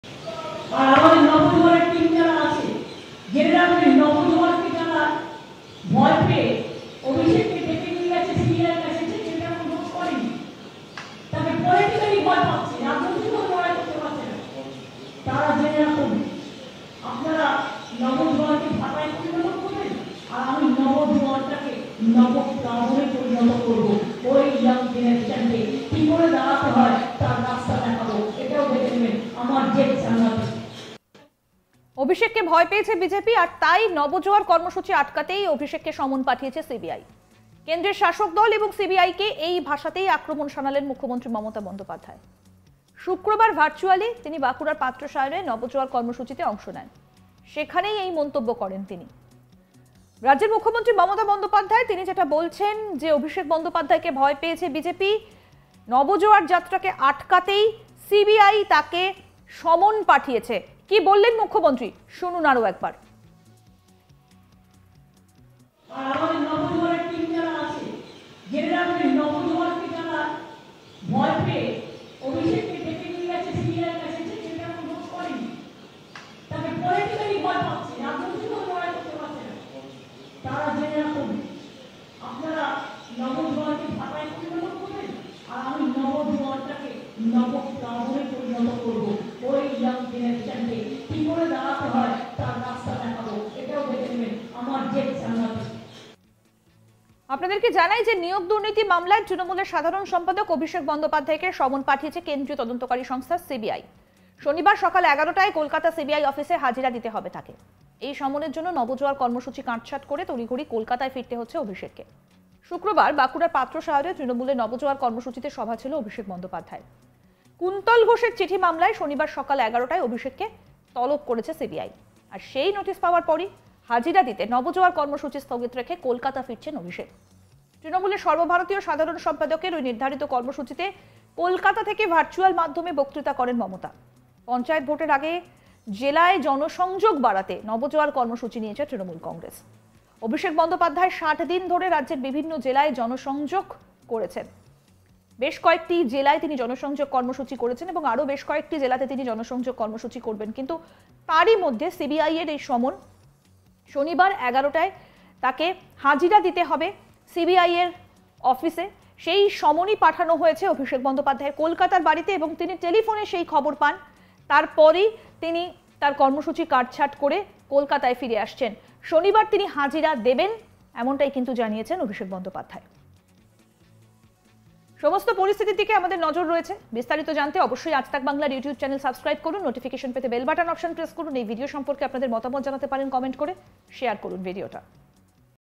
অভিষেককে ভয় পেয়েছে BJP : মমতা | Mamata Banerjee | Abhishek Banerjee | Aaj Tak Bangla
অভিষেককে ভয় পেয়েছে BJP. আর তাই নবজোয়ার কর্মসূচি আটকাতেই অভিষেককে সমন পাঠিয়েছে CBI. কেন্দ্রের শাসক দল ও CBI কে এই ভাষাতেই আক্রমণ শানালেন মুখ্যমন্ত্রী মমতা বন্দ্যোপাধ্যায়। শুক্রবার ভার্চুয়ালি তিনি বাঁকুড়ার পাত্রসায়রে নবজোয়ার কর্মসূচিতে অংশ নেন। সেখানেই এই মন্তব্য করেন তিনি।

বাইট : মমতা বন্দ্যোপাধ্যায়, মুখ্যমন্ত্রী

(BJP অভিষেককে ভয় পেয়েছে)

উল্লেখ্য, নিয়োগ দুর্নীতি মামলায় তৃণমূলের সাধারণ সম্পাদক অভিষেক বন্দ্যোপাধ্যায়কে সমন পাঠিয়েছে কেন্দ্রীয় তদন্তকারী সংস্থা CBI. শনিবার সকাল এগারোটায় কলকাতায় CBI অফিসে  হাজিরা দিতে হবে তাঁকে। এই সমনের জন্য নবজোয়ার কর্মসূচিতে কাটছাঁট করে তড়িঘড়ি কলকাতায় ফিরতে হচ্ছে অভিষেককে। শুক্রবার, বাঁকুড়ার পাত্রসায়রে তৃণমূলের ‘নবজোয়ার’ কর্মসূচিতে সভা ছিল অভিষেক বন্দ্যোপাধ্যায়ের। কুন্তল ঘোষের চিঠি মামলায় শনিবার সকাল 11 টায় অভিষেককে তলব করেছে সিবিআই। সেই নোটিস পাওয়ার পরই হাজিরা দিতে ‘নবজোয়ার’ কর্মসূচি স্থগিত রেখে কলকাতা ফিরছেন অভিষেক। তৃণমূলের সর্বভারতীয় সাধারণ সম্পাদকের ওই নির্ধারিত কর্মসূচিতে কলকাতা থেকে ভার্চুয়াল মাধ্যমে বক্তৃতা করেন মমতা। পঞ্চায়েত ভোটের আগে জেলা জেলায় জনসংযোগ বাড়াতে নবজোয়ার কর্মসূচি নিয়েছে TMC. অভিষেক বন্দ্যোপাধ্যায় 60 দিন ধরে রাজ্যের বিভিন্ন জেলায় জনসংযোগ করেছেন।